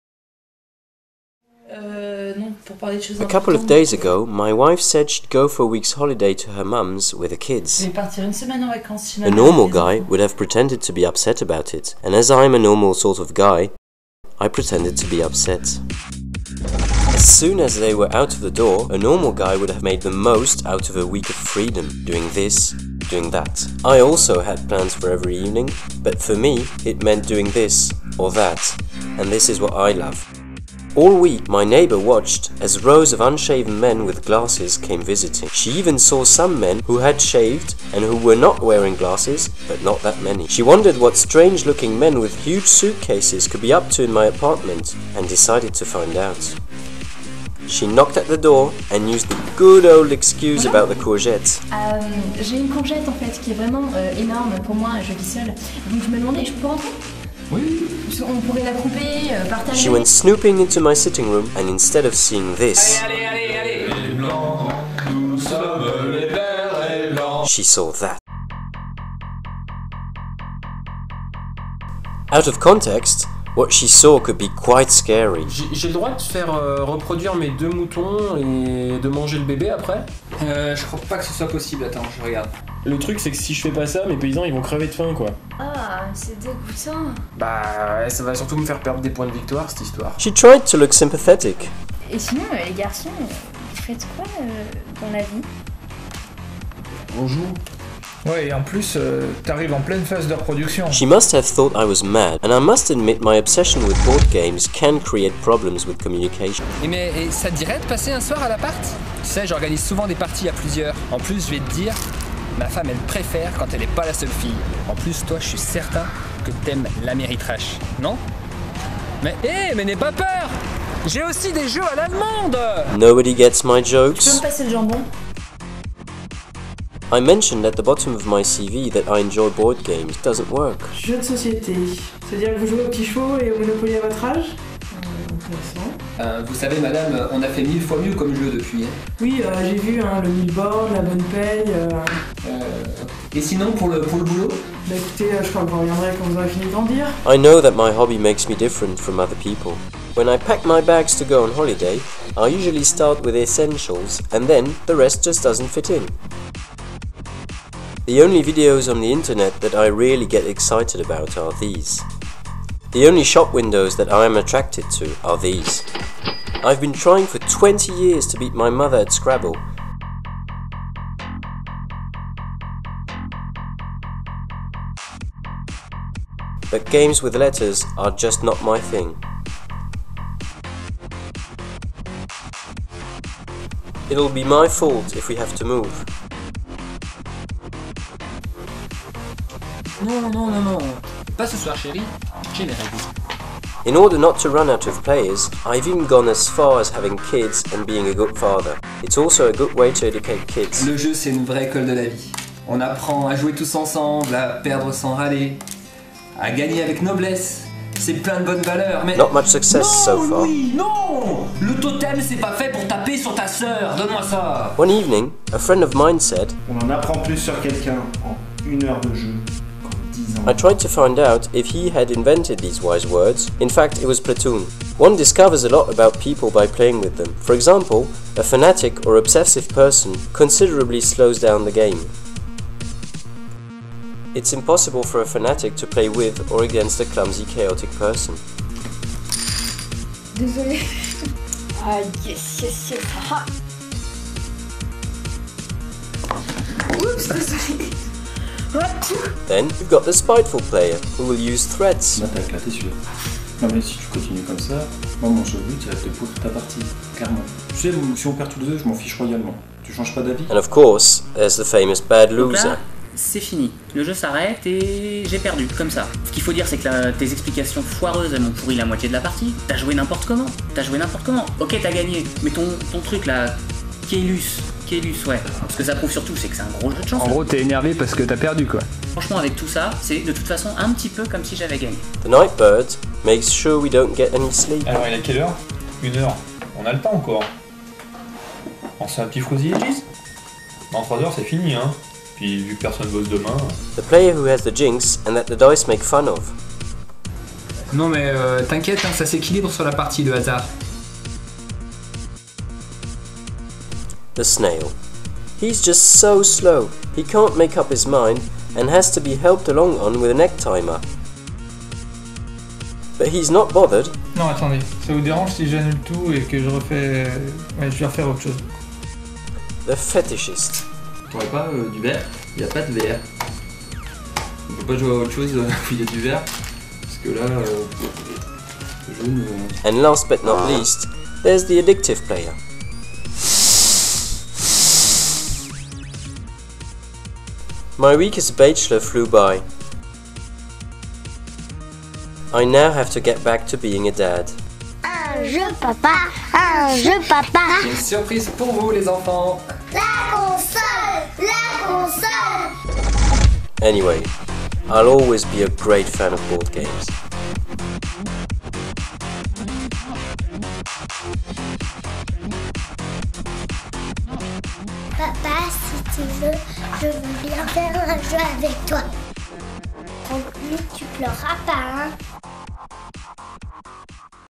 Anyway, I'm a board game geek (activate english subtitles)
This is the life of a board game geek in France...is it the same for you ? Music : Carbon Kevlar